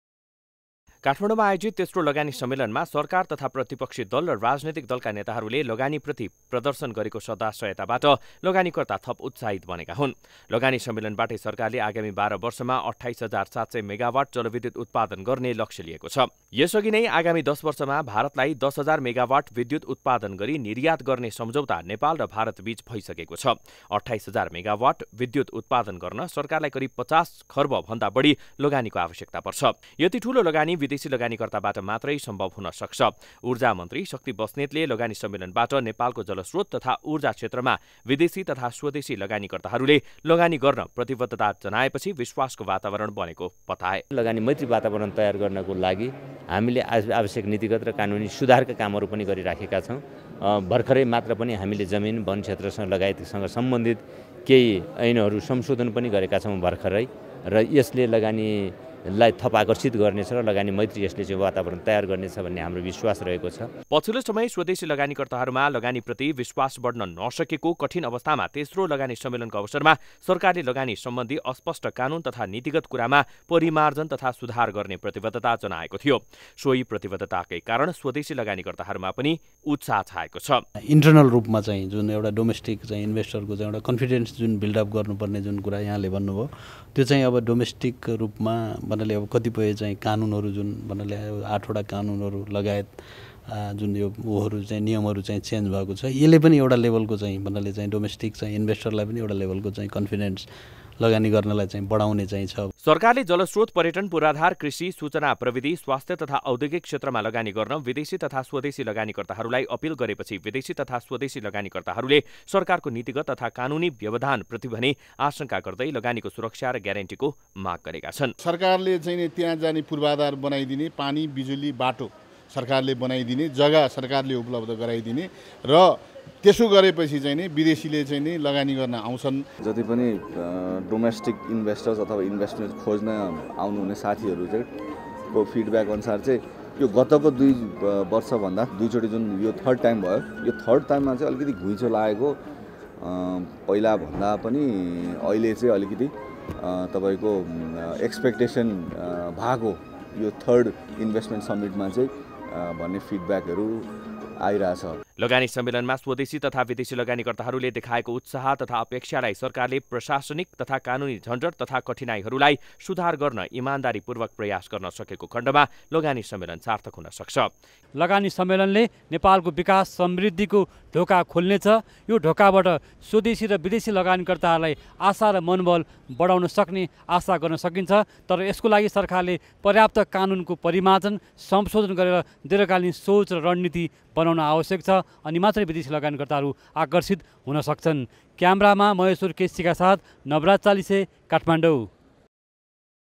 लगानी सम्मेलनमा जलविद्युतकै चर्चा, लगानीका लागि वातावरण बनेको सरकारको दावी - NEWS24 TV
सरकारले बिदेशी लगानी भित्राउन लगानी सम्वन्धि करीव एक दर्जन कानून तथा नीतिमा सुधार तथा परिमार्जन गर्ने प्रतिवद्धता लगानीकर्ताहरुको सामू गरेपछि बिदेशी तथा स्वदेशी लगानीकर्ताहरुमा उत्साह थपिएको छ । बिश्वका ५० भन्दा बढी मुलुकहरुबाट सहभागी भएका लगानीकर्ताहरुलाई सरकारले बिश्वस्त पार्दै नेपाल बैदेशिक लगानीको लागि तयार रहेको सन्देश दिए पछि बिश्वभरका लगानीकर्ताहरुको लगानीप्रतिको प्रतिबद्धता बढेको हो ।